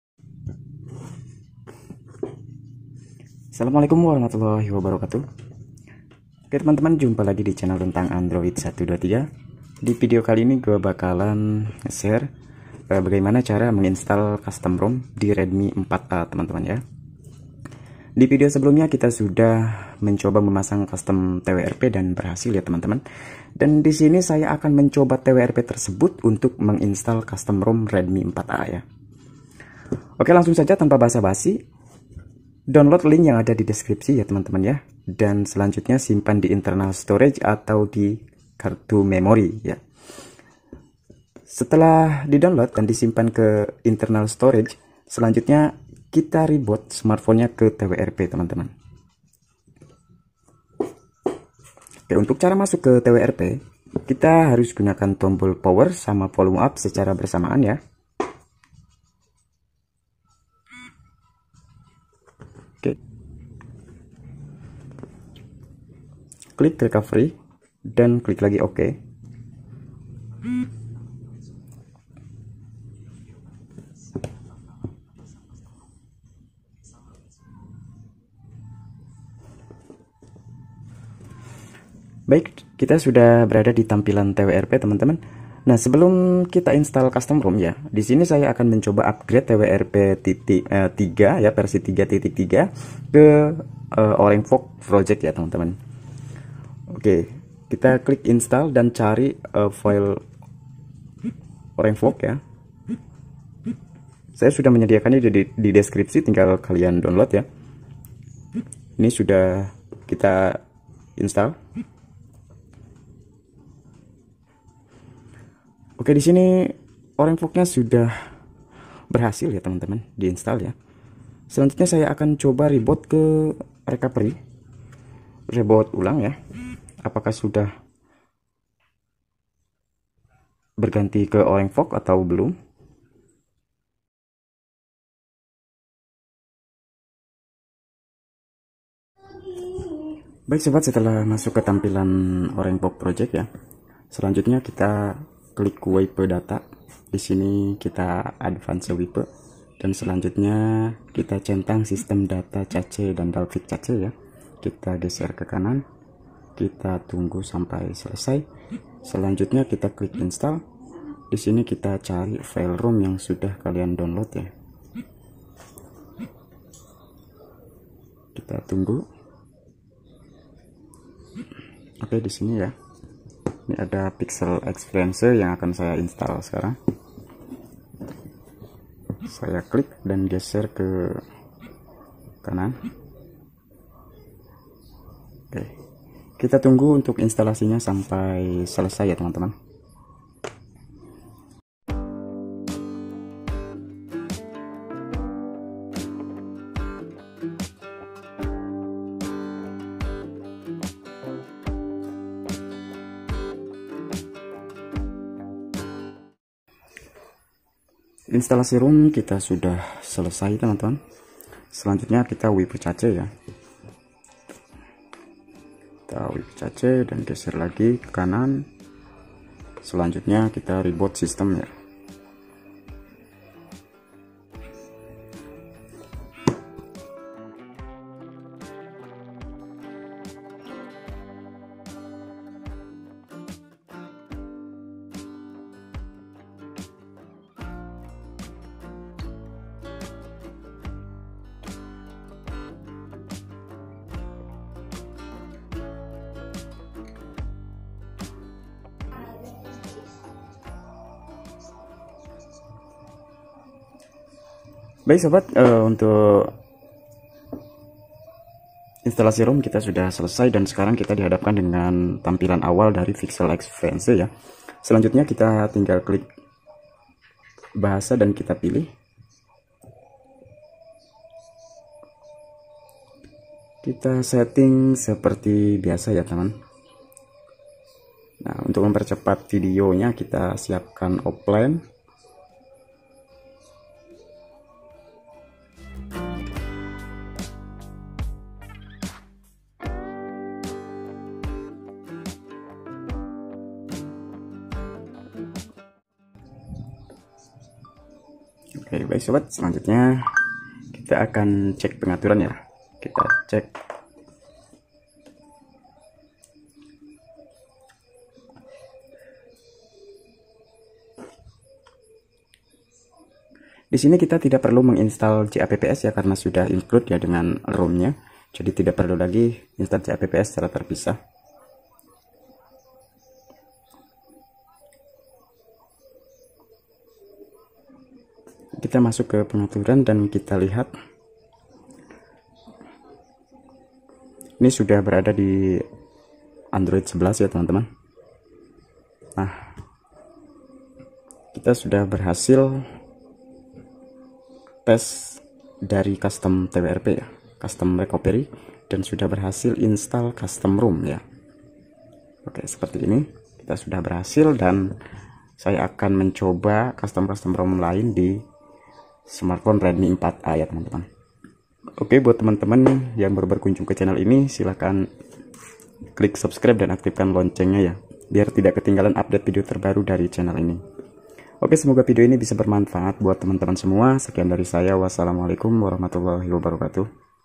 cara pasang custom rom xiaomi redmi 4a ll Custom Rom Pixel Experience Android 11 terbaru 2023
selamat datang di channel tentang android 123 divideo kali ini saya akan berbagi sedikit pengalaman saya tentang : cara pasang custom rom xiaomi redmi 4a ll Custom Rom Pixel Experience Android 11 terbaru 2023

VIDEO LAINNYA
cara pasang custom rom xiaomi redmi 4a ll Custom Rom Pixel Experience Android 11 ll PUBG lancar jaya :

0.00 : PEMBUKAAN
1.00 : REBOOT KE TWRP
1.38 : UPGRADE TWRP
2.25 : WIPE DATA SYSTEM
3.00 : INSTAL  ROM
3.45 : REBOOT SYSTEM
4.15 : SETING ROM
5.15 : INSTAL ROM SUCCES